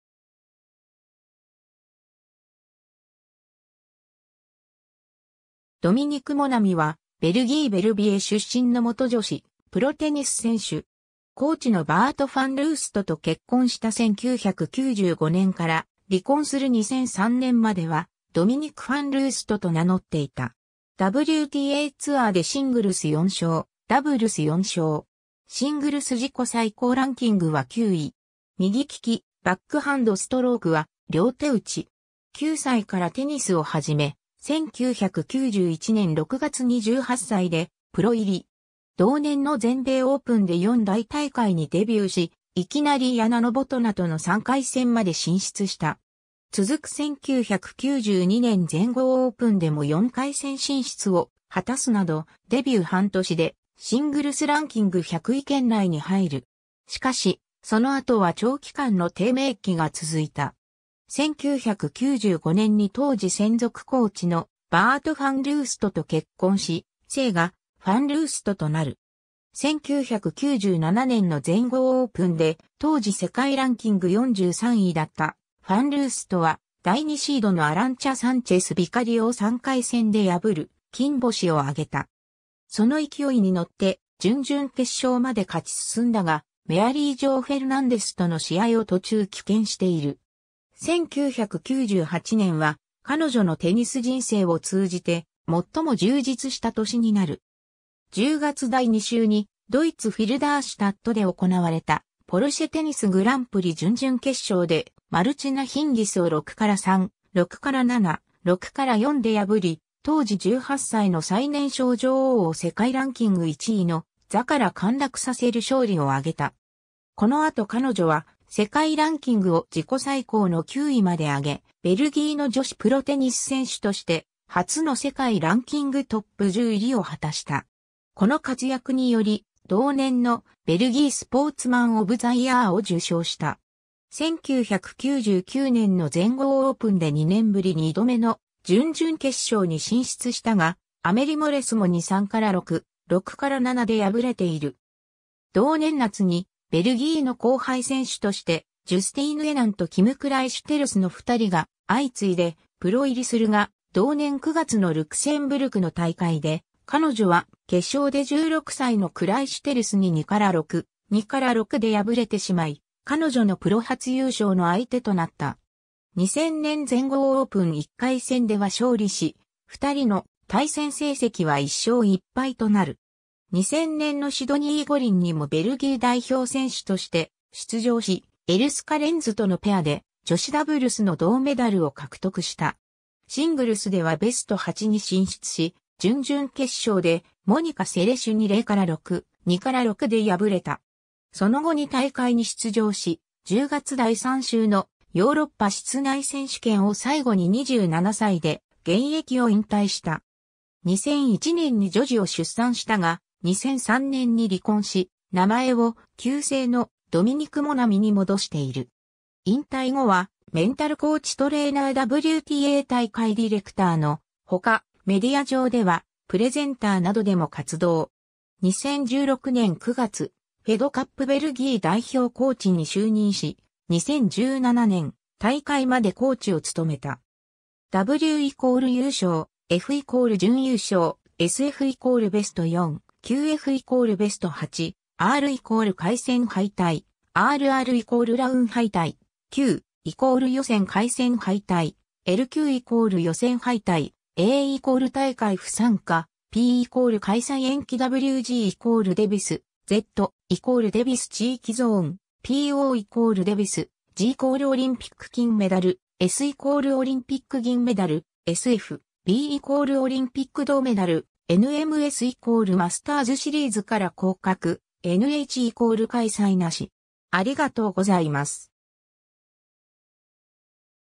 ドミニク・モナミ
ドミニク・モナミ（Dominique Monami, 1973年5月31日 - ）は、ベルギー・ヴェルヴィエ出身の元女子プロテニス選手。
コーチのバート・ファン・ルースト（Bart Van Roost）と結婚した1995年から、離婚する2003年まではドミニク・ファン・ルースト（Dominique Van Roost）と名乗っていた。
WTAツアーでシングルス4勝、ダブルス4勝。
シングルス自己最高ランキングは9位。
 9歳からテニスを始め、1991年6月に18歳でプロ入り。
同年の全米オープンで4大大会にデビューし、いきなりヤナ・ノボトナとの3回戦まで進出した。
続く1992年全豪オープンでも4回戦進出を果たすなど、デビュー半年でシングルスランキング100位圏内に入る。
しかし、その後は長期間の低迷期が続いた。
 1995年に当時専属コーチのバート・ファン・ルースト（Bart Van Roost）と結婚し、姓がファン・ルースト（Van Roost）となる。
 1997年の全豪オープンで、当時世界ランキング43位だったファン・ルーストは、第2シードのアランチャ・サンチェス・ビカリオを3回戦で破る金星を挙げた。
その勢いに乗って準々決勝まで勝ち進んだが、メアリー・ジョー・フェルナンデスとの試合を途中棄権している。
 1998年は彼女のテニス人生を通じて、最も充実した年になる。
10月第2週にドイツ・フィルダーシュタットで行われた「ポルシェ・テニス・グランプリ」準々決勝でマルチナ・ヒンギスを 6-3, 6-7, 6-4 で破り、当時18歳の最年少女王を世界ランキング1位の座から陥落させる勝利を挙げた。
この後彼女は世界ランキングを自己最高の9位まで上げ、ベルギーの女子プロテニス選手として初の世界ランキングトップ10入りを果たした。
この活躍により、同年の『ベルギー スポーツマンオブザイヤー』を受賞した。
 1999年の全豪オープンで2年ぶり2度目の準々決勝に進出したが、アメリ・モレスモに 3-6, 6-7 で敗れている。
同年夏に、ベルギーの後輩選手としてジュスティーヌ・エナンとキム・クライシュテルスの2人が相次いでプロ入りするが、同年9月のルクセンブルクの大会で、彼女は決勝で16歳のクライシュテルスに 2-6, 2-6 で敗れてしまい、彼女のプロ初優勝の相手となった。
2000年全豪オープン1回戦では勝利し、2人の対戦成績は1勝1敗となる。
（エナンとは対戦なし） 2000年のシドニー五輪にもベルギー代表選手として出場し、エルス・カレンズとのペアで女子ダブルスの銅メダルを獲得した。
シングルスではベスト8に進出し、準々決勝でモニカ・セレシュに 0-6, 2-6 で敗れた。
その後2大会に出場し、10月第3週の「ヨーロッパ室内選手権」（スイス・チューリヒ）を最後に27歳で現役を引退した。
 2001年に女児を出産したが、2003年に離婚し、名前を旧姓の「ドミニク・モナミ」に戻している。
 引退後はメンタルコーチ・トレーナー・WTA大会ディレクターのほか、メディア上ではプレゼンターなどでも活動。
2016年9月、フェドカップベルギー代表コーチに就任し、2017年大会までコーチを務めた。